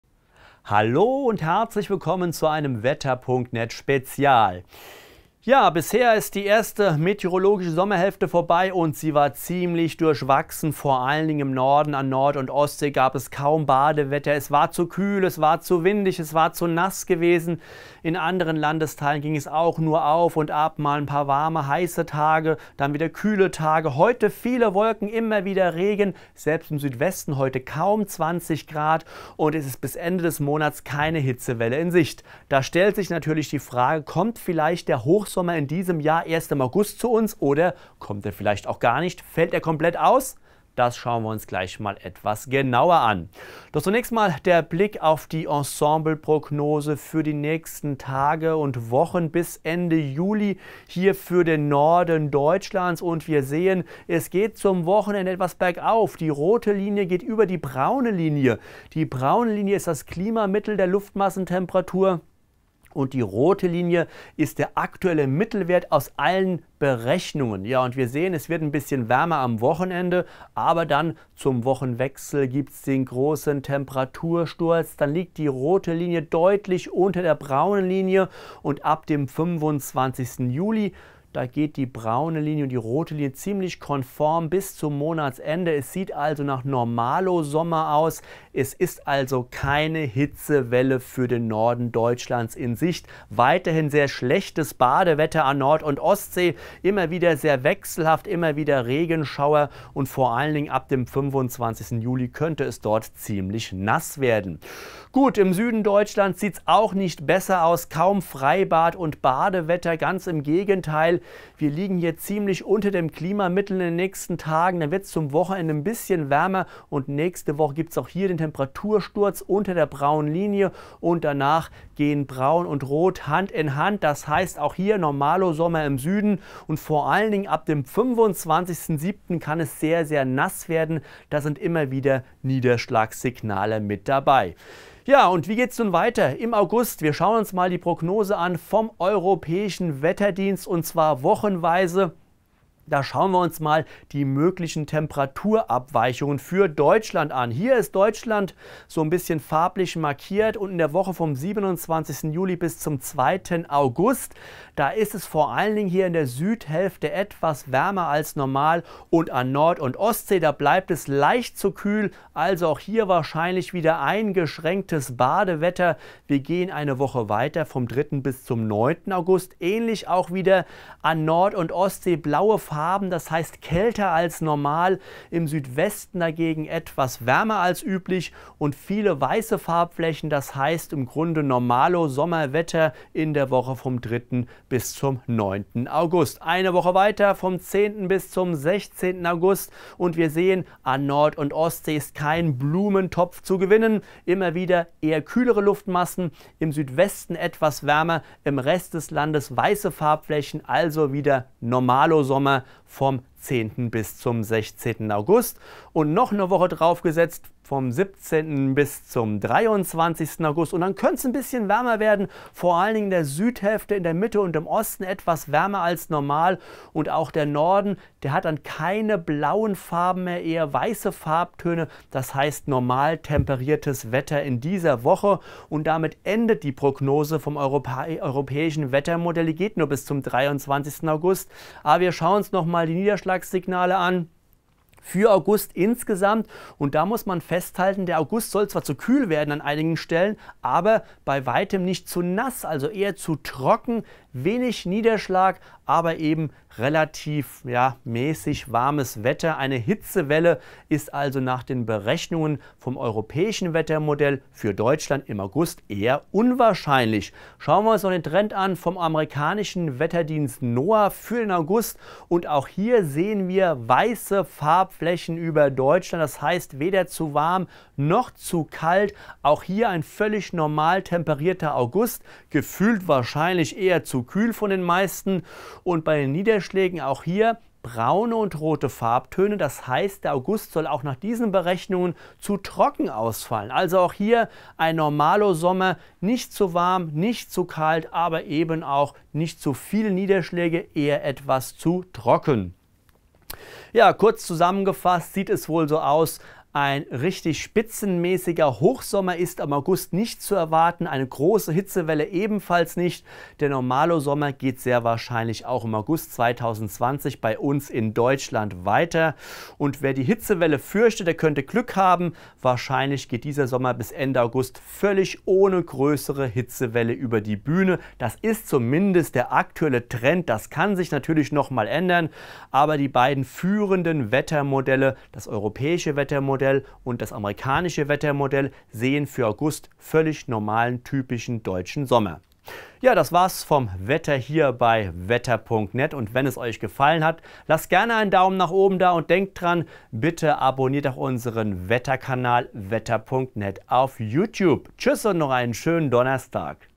Fallen Hochsommer und Hitze dieses Jahr komplett aus? Der aktuelle Trend für August 2020!
Auf vielfachen Wunsch gucken wir heute mal ein wenig in den August hinein. Kommt da der Hochsommer? Wird es heiß? Oder fällt der Hochsommer in diesem Jahr komplett aus? Das sagt die aktuelle Monatsprognose vom europäischen und US-Wetterdienst für Deutschland...

Das aktuelle Deutschlandwetter vom
 16. Juli 2020.